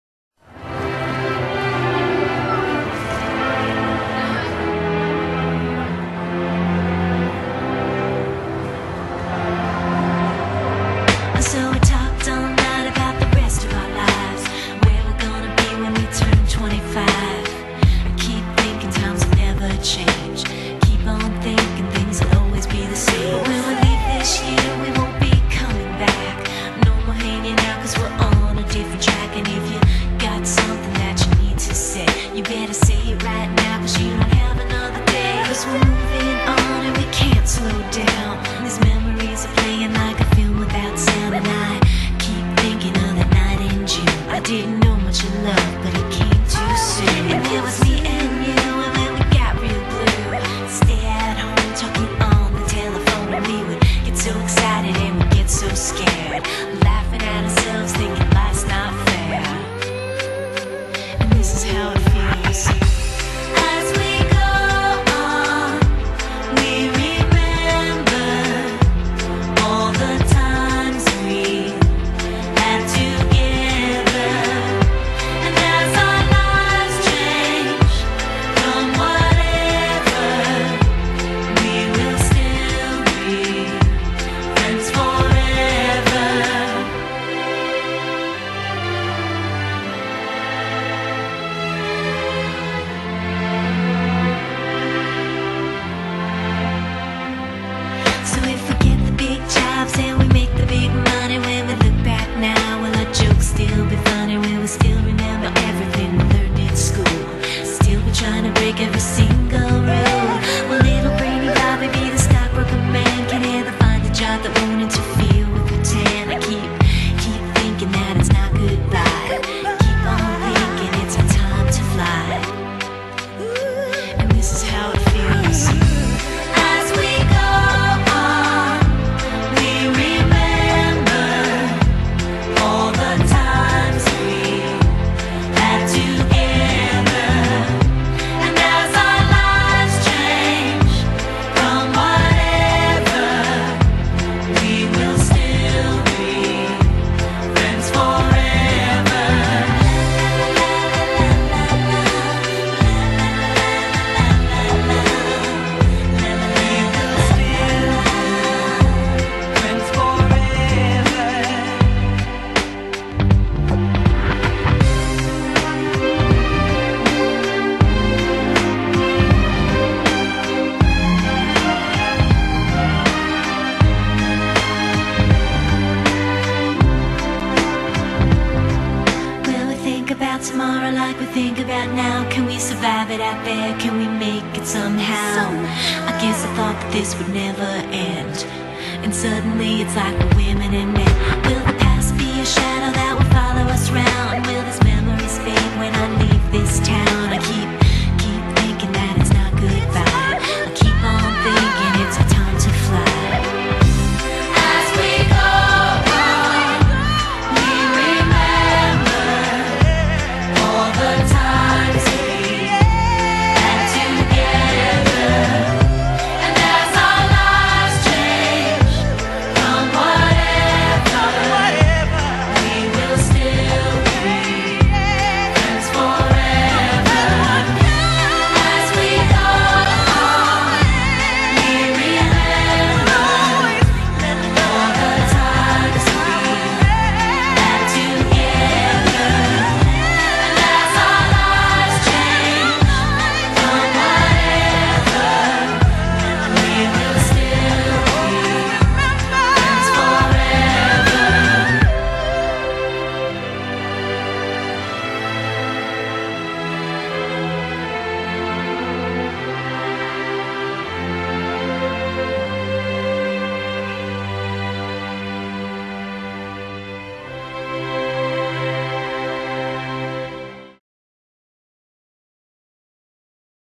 Class of 2017 (Cherbourg Campus)
A compilation of photos and videos taken during your time at Crestwood College.

Best of luck for the future!